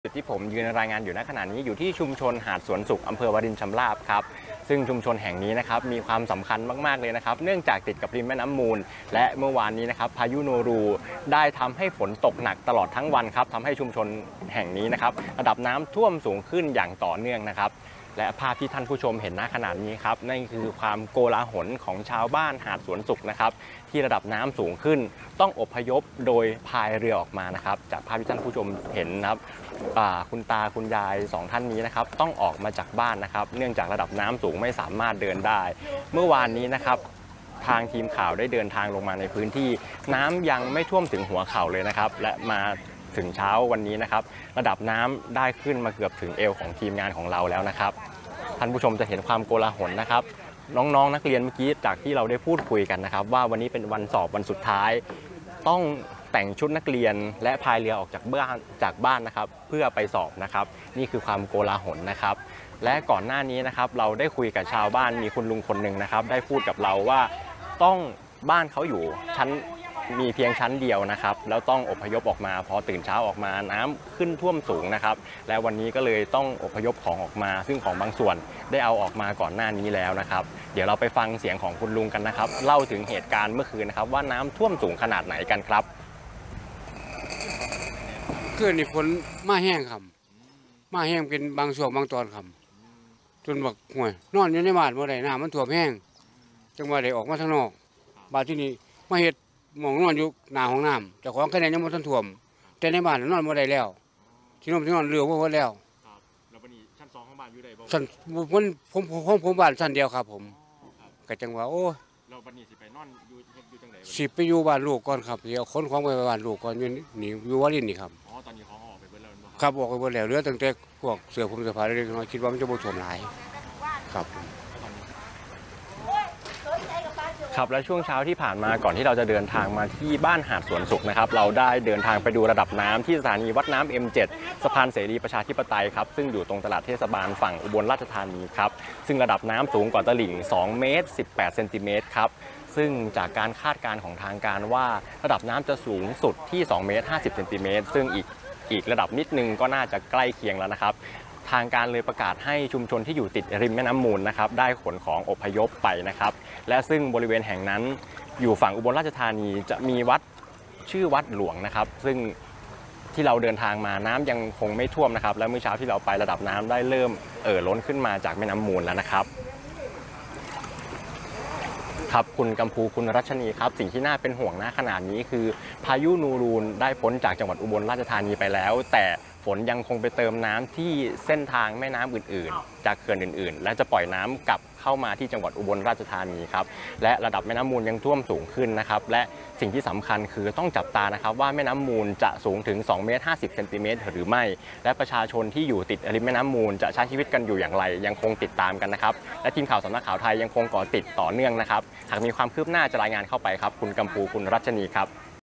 อุบลฯ อ่วมฤทธิ์พายุ “โนรู” ชาวบ้านริมน้ำมูลอพยพหนี
สถานการณ์น้ำท่วมในพื้นที่ จ.อุบลราชธานี จากอิทธิพลของพายุ “โนรู” เช้าวันนี้ (29 ก.ย.) ที่ชุมชนหาดสวนสุข อ.วารินชำราบ ซึ่งอยู่ติดกับแม่น้ำมูล ระดับน้ำท่วมเพิ่มสูงขึ้นต่อเนื่อง หลังวานนี้ฝนตกหนักตลอดทั้งวัน ชาวบ้านพากันพายเรืออพยพออกจากบ้านเรือนอย่างโกลาหล